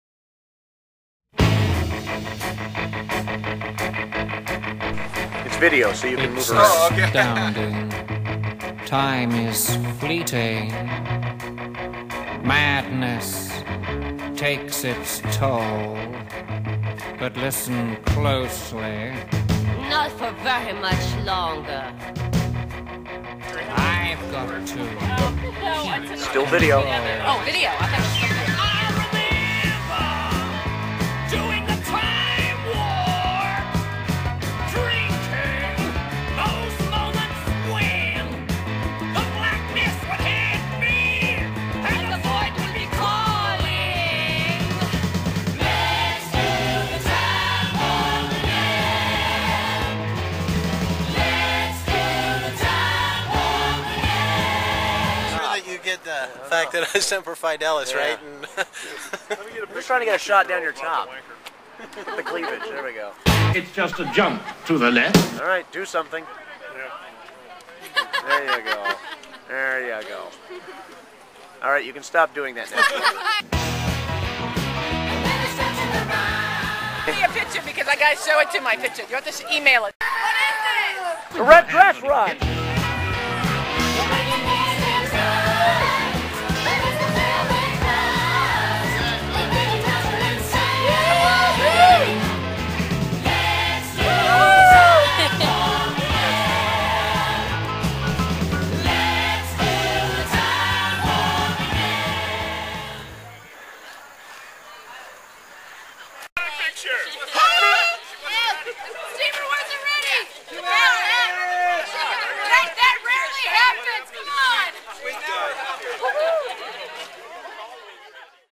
Cincinnati/Dayton Red Dress Run 08
Cincinnati and Dayton Hash House Harriers annual Red Dress Run, in Dayton, Ohio, Sept 20, 2008.